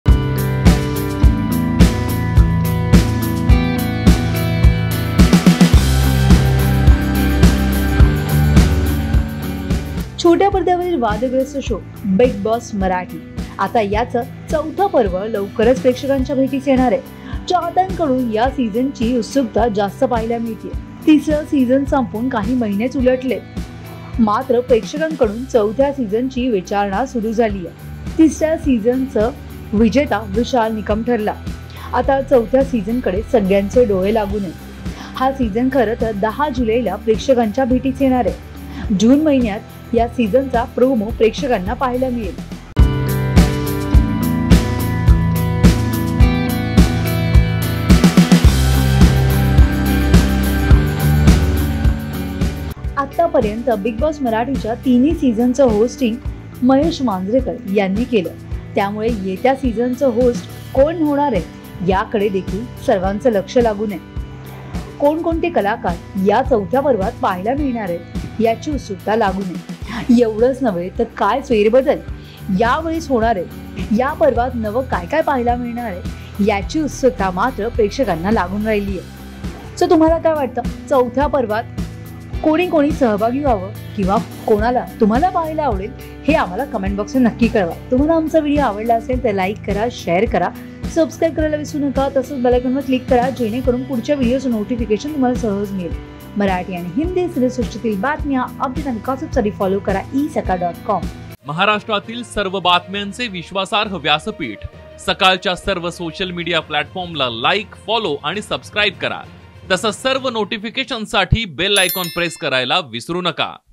Bigg Boss Marathi Season 4 Coming Soon : ‘बिग बॉस मराठी’चं चौथ पर्व | Sakal Media
छोट्या पडद्यावरील वादग्रस्त शो बिग बॉस मराठीचा सिझन ४ लवकरच प्रेक्षकांच्या भेटिस येणार असून चाहत्यांकडून चौथा सिझनची उत्सुकता पाहिला मिळते...तिसरा सिझन सुपर हिट ठरला तिस-या सिझनचा विजेता विशाल निकम ठरला... आता चौथा सिझनकडे सगळ्याचे डोळे लागून आहेत...